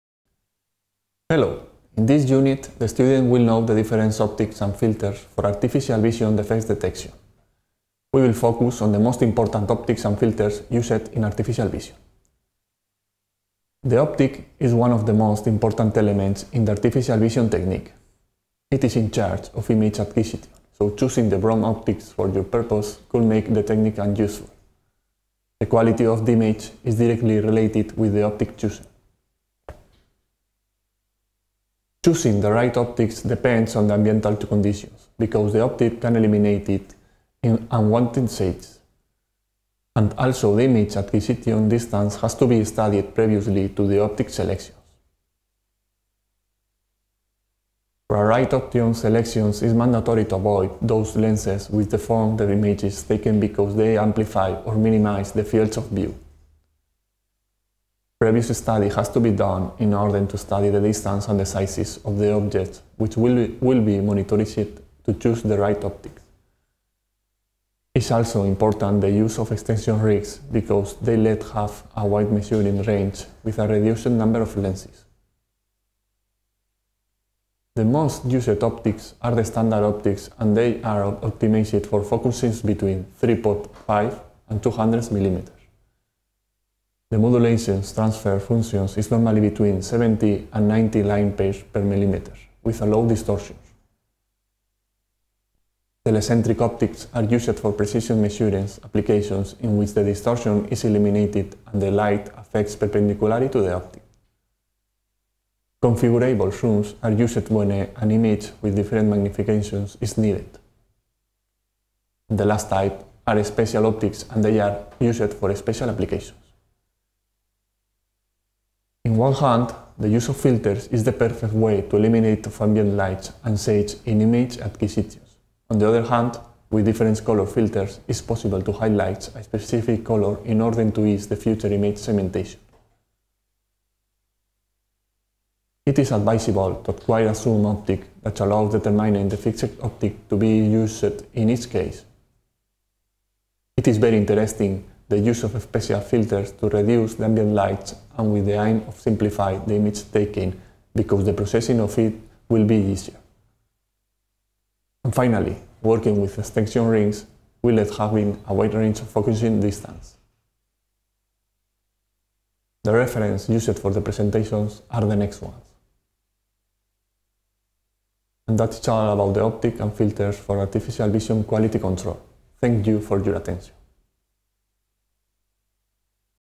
Optics and filters for artificial vision quality control | 54/87 | UPV
Título: Optics and filters for artificial vision quality control

Descripción automática: In this video, the speaker discusses the importance of selecting appropriate optical filters for artificial vision systems, particularly in the context of face detection. The quality of the images captured by these systems is crucially influenced by the choice of optics, which are responsible for image applications. Choosing the right optics requires considering environmental conditions and the need to eliminate distortions.

The correct optics selection must take into account the distance and size of the objects to be imaged, optimizing the field of view. Extension rings are highlighted for their usefulness in adjusting focus range without adding multiple lenses. The most commonly used standard optics in artificial vision have a resolution focus range between 3.5 to 200 mm and modulation transfer functions of 70 to 90 lines per mm, with low distortion preferred for precision measurements.

The speaker also emphasizes the role of optical filters in enhancing image quality. Filters can remove glare from lights, adapt to changing light conditions, or emphasize specific colors to facilitate image segmentation. Polarization filters, in particular, can reduce glare and simplify image capture and processing. For versatile applications requiring a wide focus range, a particular set of filters may be recommended.

The video culminates with a reminder of the upcoming presentations on optical filters for artificial vision and a thank-you message to the audience.

Autor/a: Valor Cerdá Miguel

Curso: Este vídeo es el 54/87 del curso MOOC Advanced technologies for textile quality control. Artificial Vision.